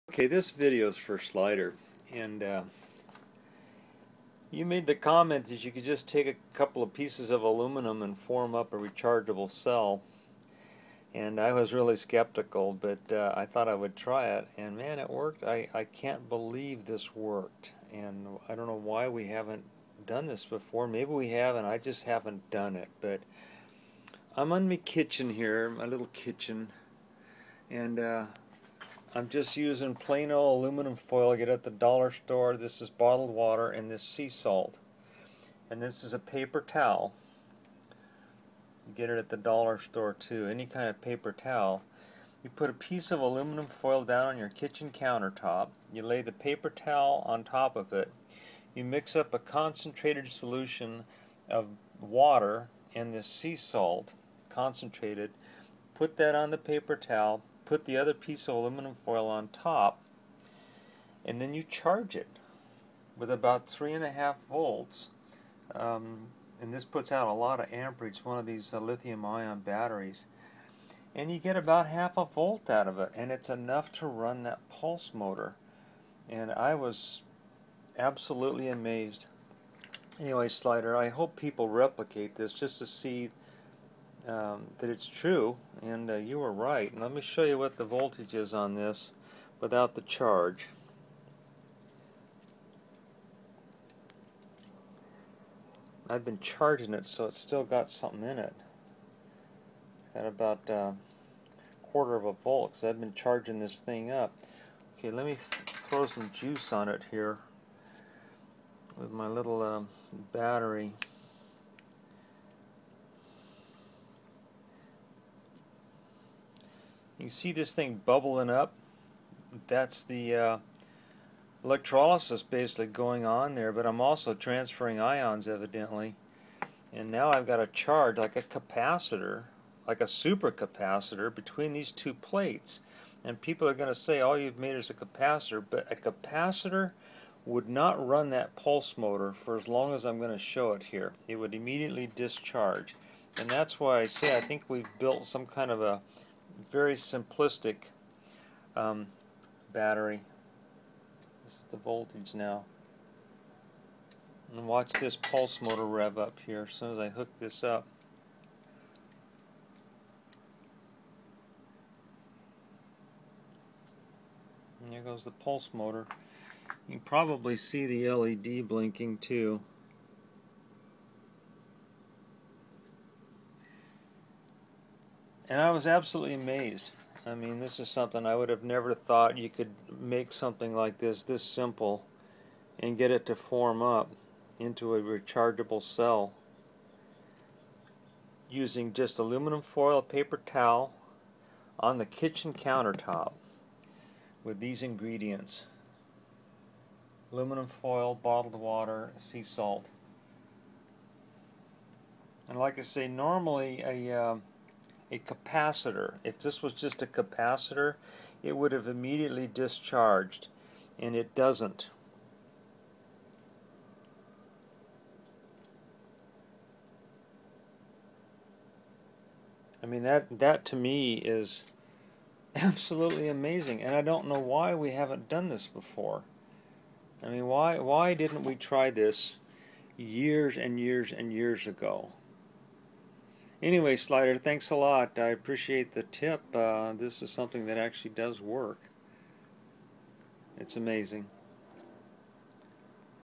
Amazing Simple Battery
This a simple battery that was suggested by Slider2732.  It is just two pieces of aluminum foil with a salt water electrolyte. When charged it becomes a cell with a potential of about half a volt.  There is enough power to run a joule thief led light or a low power pulse motor.  This battery will run a dollar store solar waving toy without the solar panel.